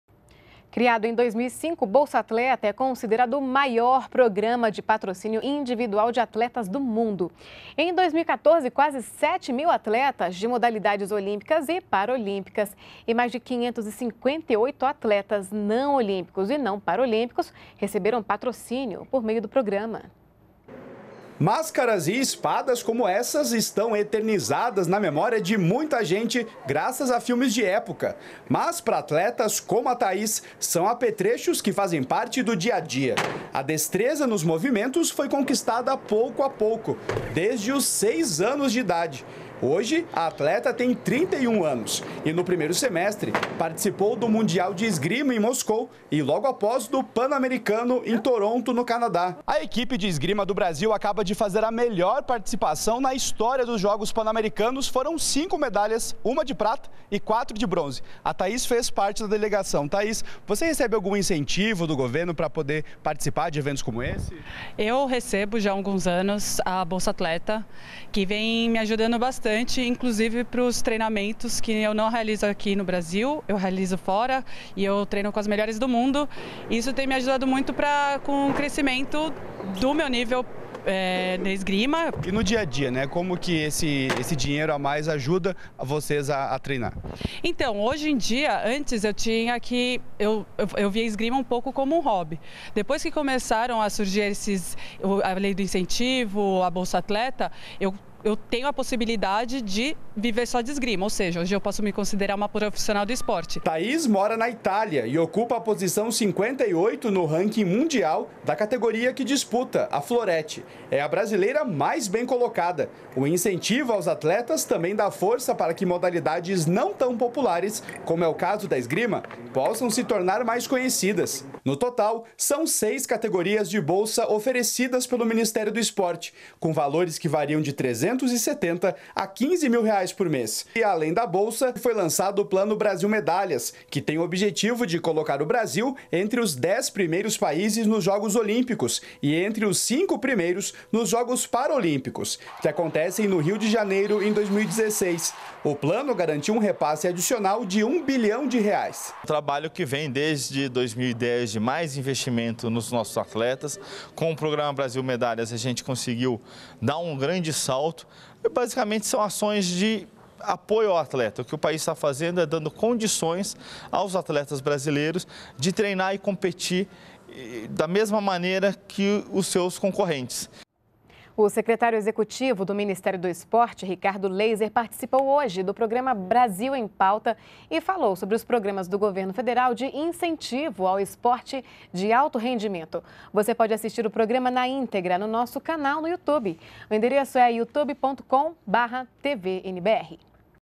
Programa Bolsa-Atleta apoiou quase 7 mil competidores olímpicos de alto rendimento
NBR NOTÍCIAS - 04.08.15: O Bolsa-Atleta é o maior programa de patrocínio individual para atletas no mundo. O programa federal patrocina os atletas com alto rendimento que atingem bons resultados nas competições nacionais e internacionais de sua modalidade. São ofertadas pelo Ministério do Esporte bolsas em cinco categorias,  Atleta de Base, Estudantil, Nacional, Internacional e Olímpico/Paraolímpico.